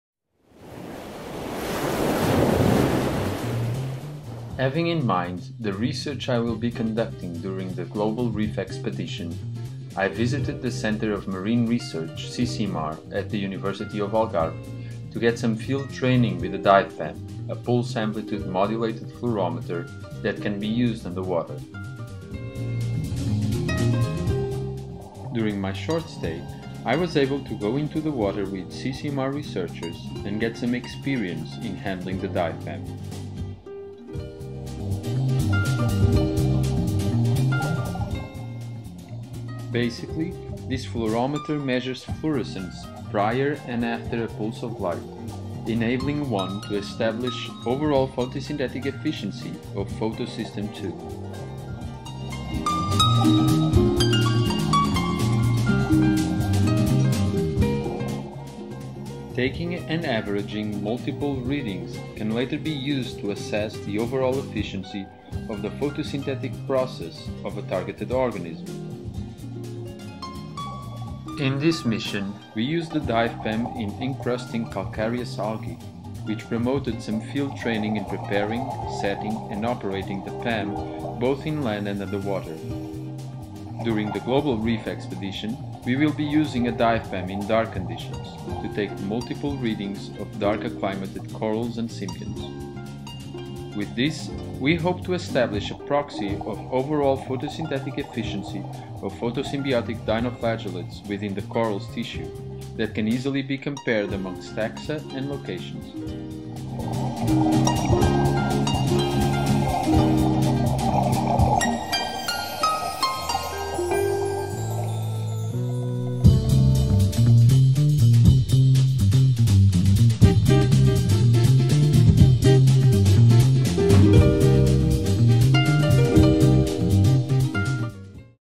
How Much Does Coral Glow? João Monteiro uses a PAM Fluorometer for Reef Research
João Monteiro will use an underwater PAM fluorometer to measure photosynthetic efficiency of reef ecosystems for his research as part of the Global Reef Expedition.

Video by João Monteiro, Khaled bin Sultan Living Oceans Foundation